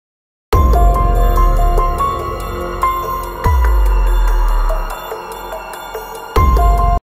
Triangle Films LOGO
An attempt at an 80s style Movie company logo.